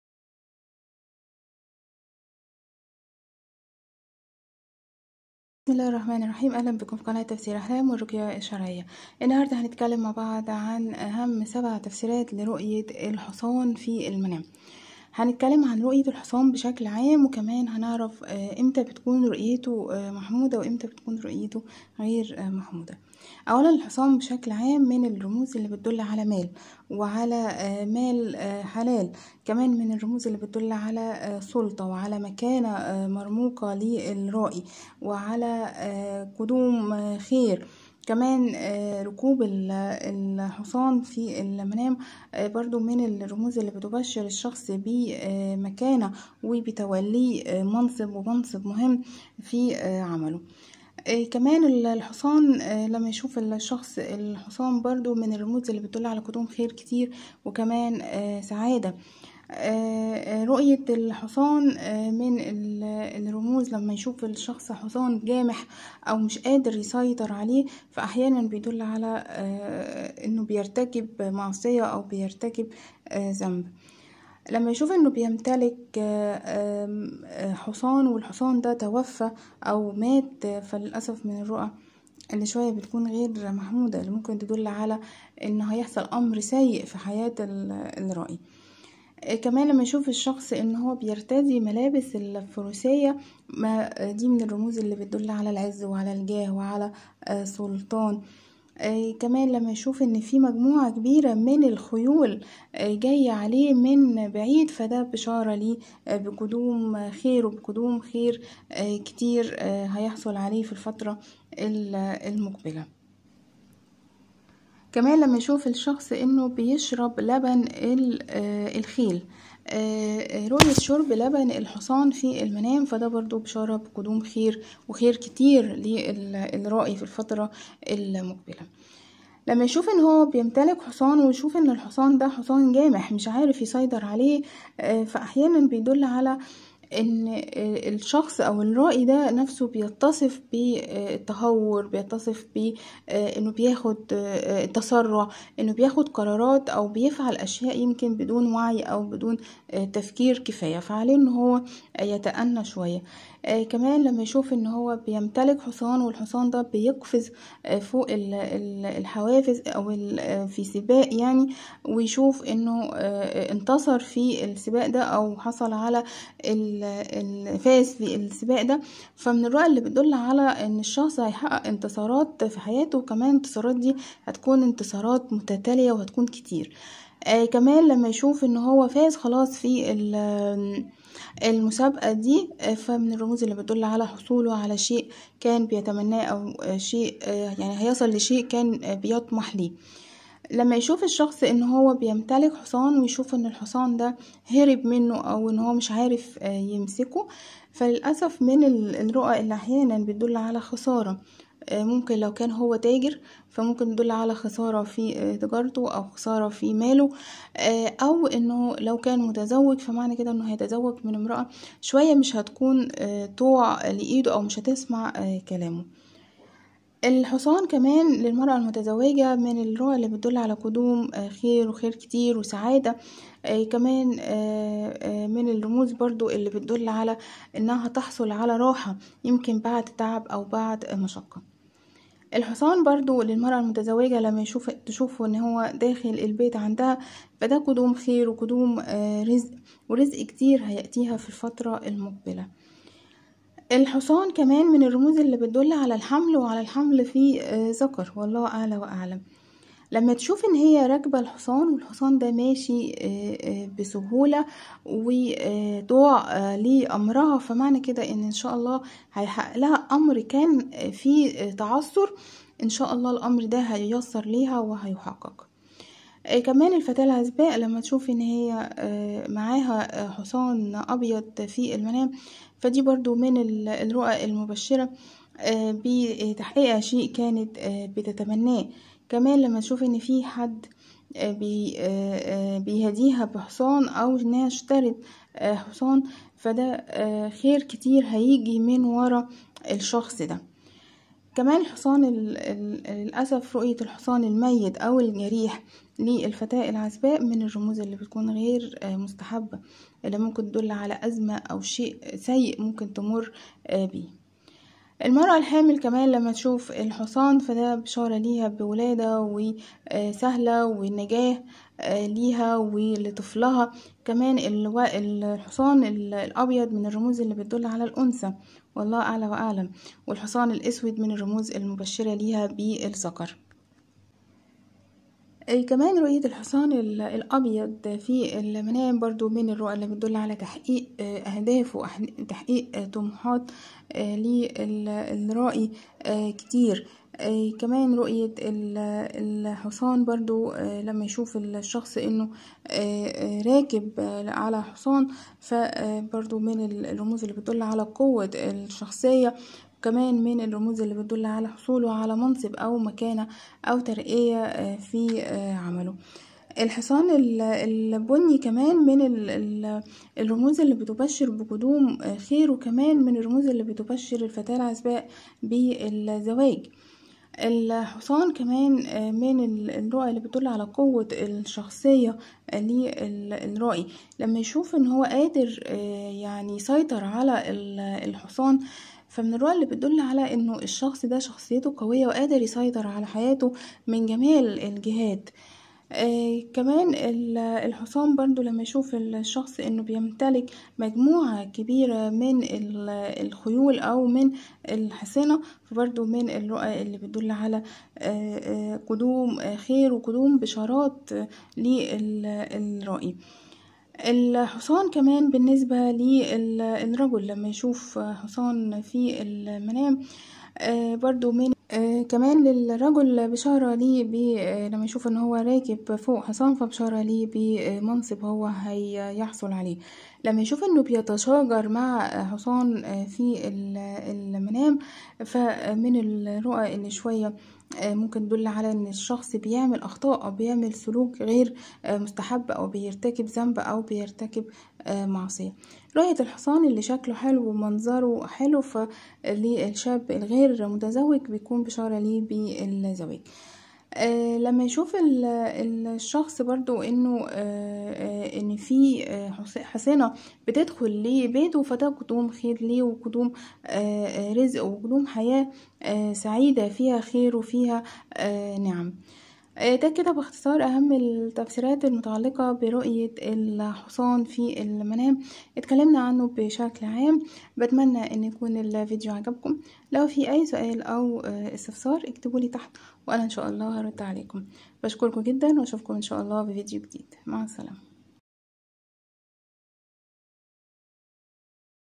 تفسير حلم رؤية الحصان في المنام ما معنى رؤية الحصان وخيول بالمنام |تفسير الاحلام فاطمة الزهراء
يتناول الفيديو شرح لاهم التفسيرات متعلقة برؤية الحصان في المنام لابن سيرين للعزباء للمتزوجة للحامل للرجل للمطلقة